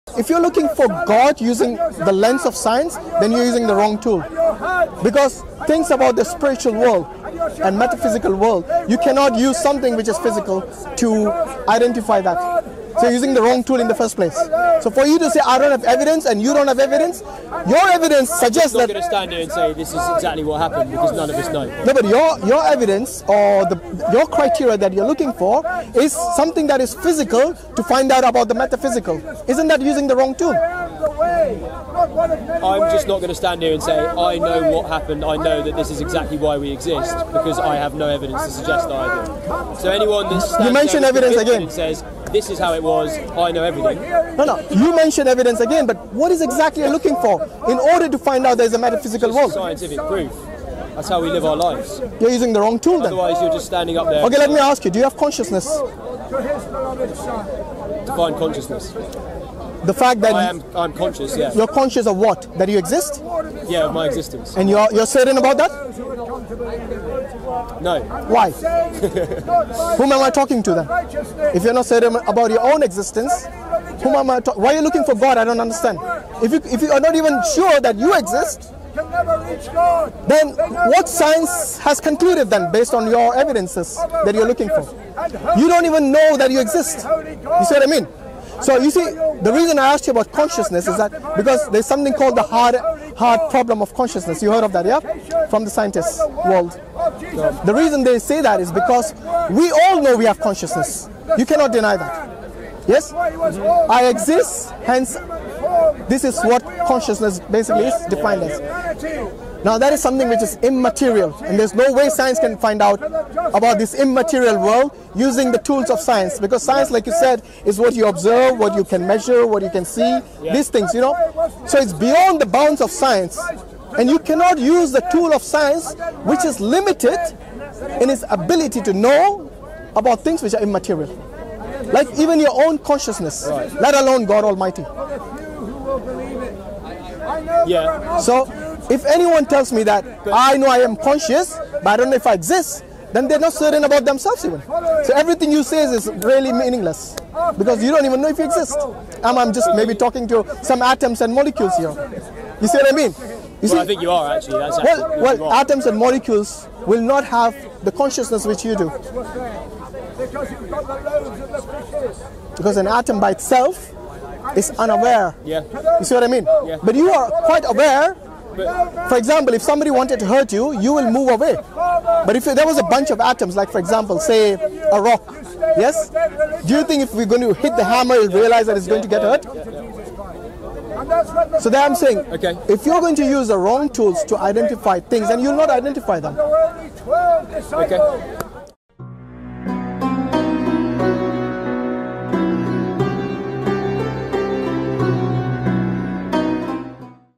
Ateis Keliru Kewujudan Diri Apatah Lagi Kewujudan Tuhan.
Bro Hashim berbincang dengan seorang ateis yang inginkan bukti sains untuk kewujudan Tuhan. Bro Hashim membalas balik dengan satu solan yang pasti membuatkan gologan ateis terkelu.